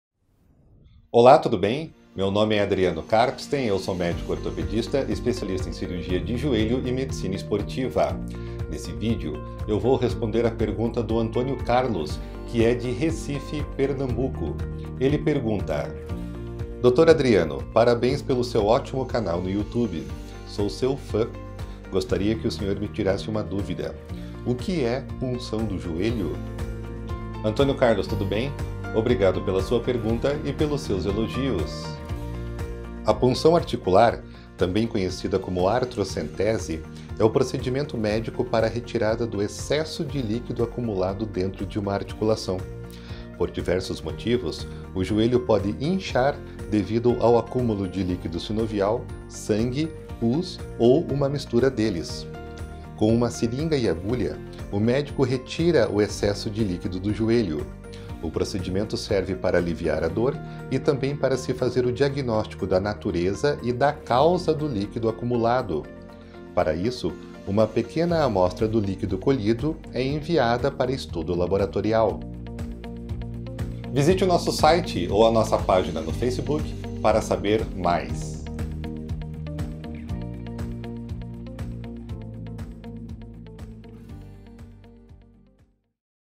O QUE É PUNÇÃO DO JOELHO?
O DR. ADRIANO KARPSTEIN, médico ortopedista especialista em Cirurgia de Joelho e Medicina Esportiva, explica O QUE É PUNÇÃO DO JOELHO.

CLÍNICA DO JOELHO - DR. ADRIANO KARPSTEIN
CRM-PR 15.995 — RQE 12.438 — RQE 12.555

Olá, tudo bem? Meu nome é ADRIANO KARPSTEIN. Eu sou MÉDICO ORTOPEDISTA ESPECIALISTA EM CIRURGIA DE JOELHO e MEDICINA ESPORTIVA.

Nesse vídeo eu vou responder a pergunta do Antônio Carlos, que é de Recife, Pernambuco. Ele pergunta:

“ Dr. Adriano, parabéns pelo seu ótimo canal no YouTube. Sou seu fã! Gostaria que o senhor me tirasse uma dúvida. O que é punção do joelho? “

Antônio Carlos, tudo bem? Obrigado pela sua pergunta e pelos seus elogios.

A punção articular, também conhecida como artrocentese, é o procedimento médico para retirada do excesso de líquido acumulado dentro de uma articulação. Por diversos motivos, o joelho pode inchar devido ao acúmulo de líquido sinovial, sangue, pus ou uma mistura deles. Com uma seringa e agulha, o médico retira o excesso de líquido do joelho. O procedimento serve para aliviar a dor e também para se fazer o diagnóstico da natureza e da causa do líquido acumulado. Para isso, uma pequena amostra do líquido colhido é enviada para estudo laboratorial.

Visite o nosso site ou a nossa página no Facebook para saber mais.